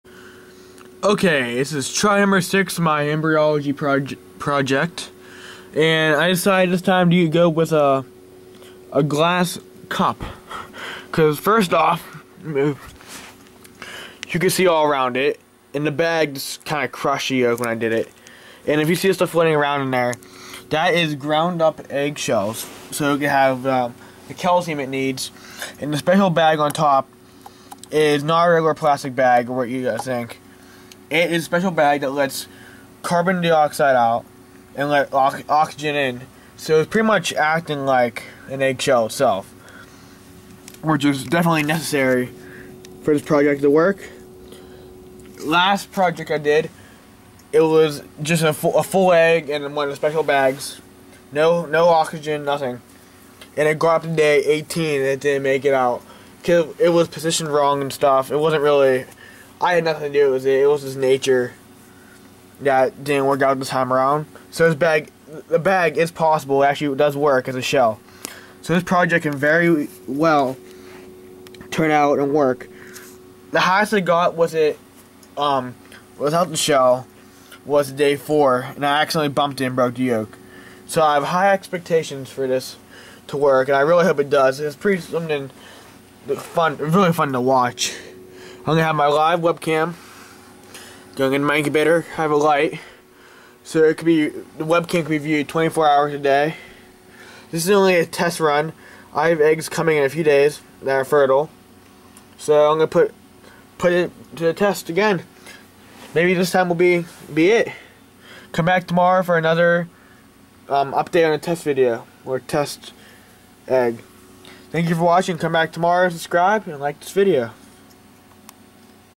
New Way To Hatch And Grow An Embryo Try #6 (Test Day 1)
I had an egg in the Kordon breathing bag that made it to day 19, so close to hatch.

It took me 5 tries to even get an egg to hatch when I first started hatching eggs, now I hatched 200+

Im trying to find a new way to hatch and grow an embryo.

Project Started back in 2009.

I am going to try  Kordon breathing bag... it should allow gas exchange but maintain moisture level. They are used for shipping fish and they have tested them with some fish for several weeks. The bag is a membrane that allows oxygen in, CO2 out, and keeps the liquid inside.

What im doing is a using a butter Containers to hold the yoke and white and using Kordon breathing bag to cover it, and putting the egg shell on the white so the the growing embryo can get calcium to grow.

The next is Im just using the bag to hold the yoke and white to see if it will grow.

Last Im just putting an egg in the bag to see if it can live up to term, and to see if the bag can receive oxygen to see if the embryo can live under the perfect condition of the egg, with out me doing anything to it. So this test is just to see if the bag can allows oxygen in, CO2 out and to see if the embryo can grow in the bag and inside its own shell. when it gets late into hatching I will poke holes in the bag so it can get more oxygen